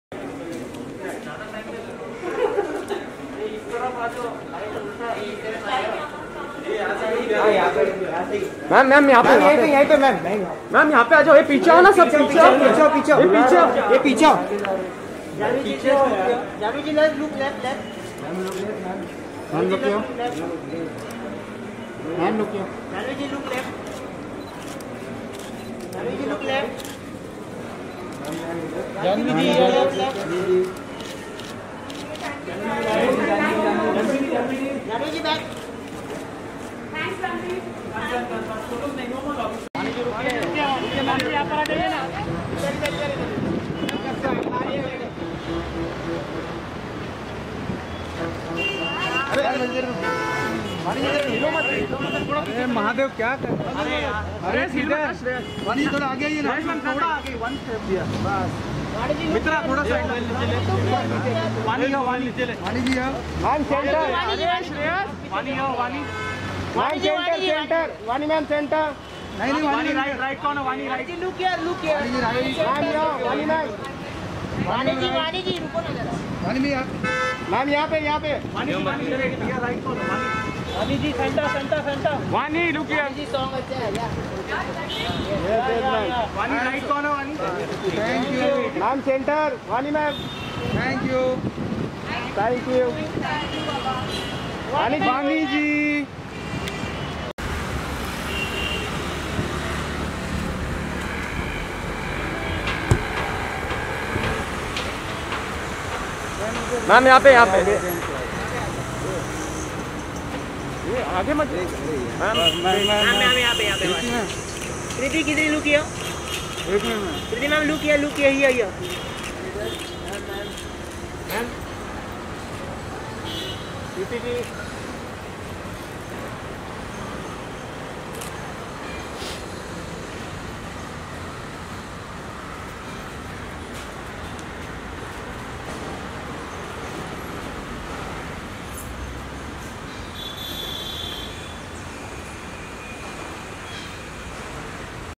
Hot transformation of Jhanvi Kapoor seen in pink dress when she snapped at Juhu.
Jhanvi Kapoor snapped at Juhu she looks so hot in her pink outfit.